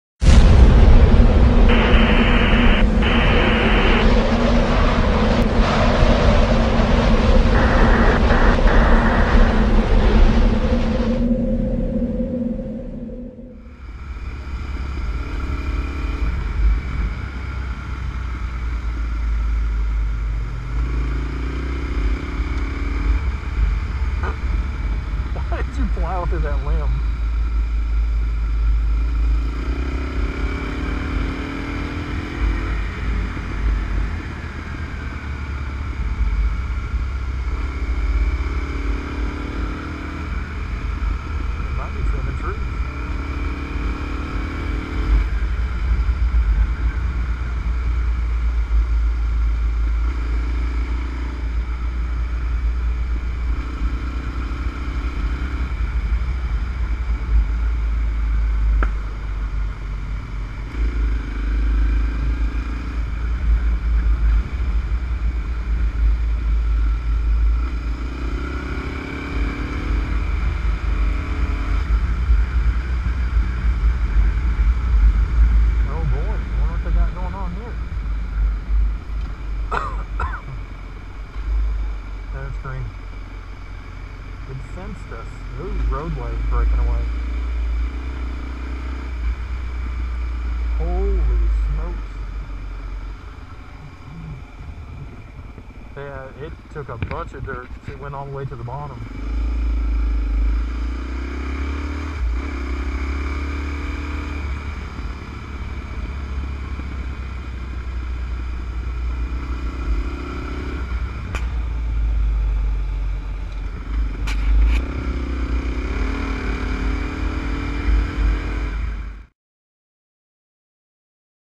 "East Tennessee Adventure Rides" Highway 32 (Road Damage) 5/11/17
Looks like whoever started to sawmill this area went in and cut a road below the highway and caused the bank to shift downhill.